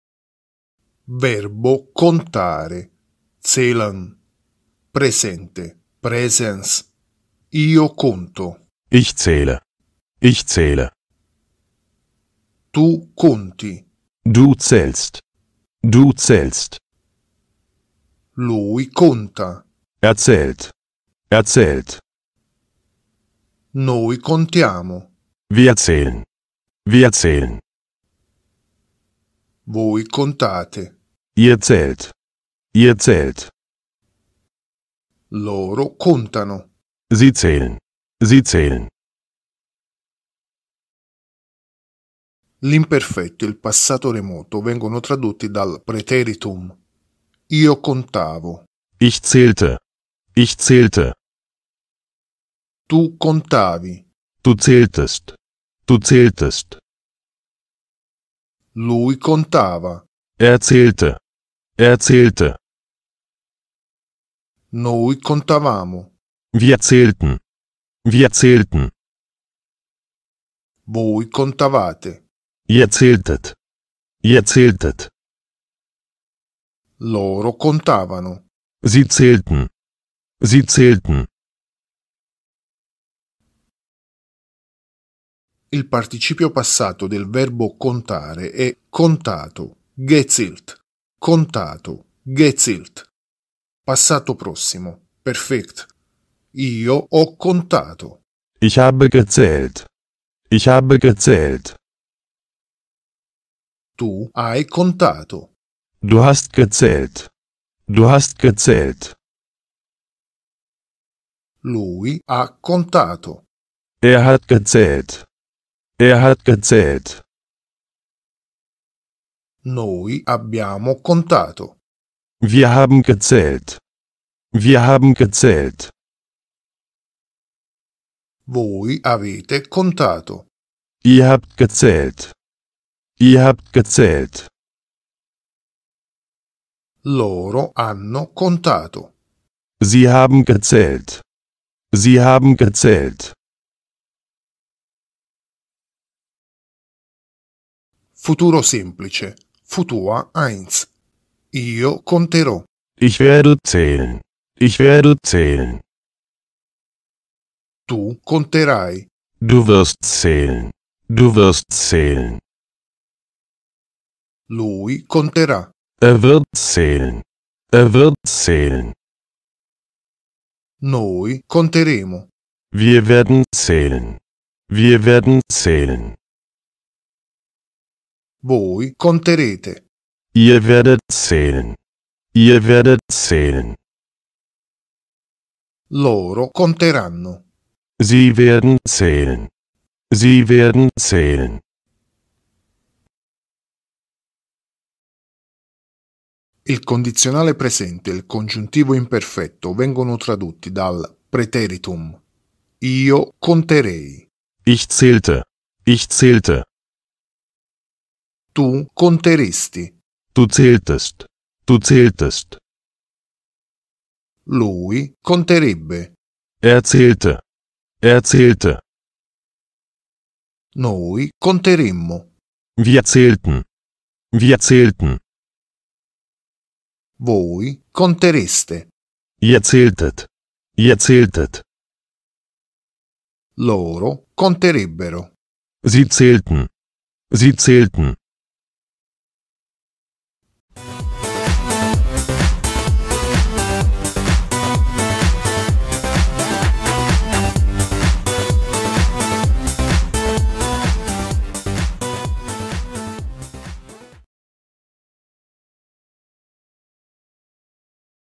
tedescofacile​ TEDESCO FACILE #260 -- Il verbo contare -- ZÄHLEN
La coniugazione del verbo contare -- ZÄHLEN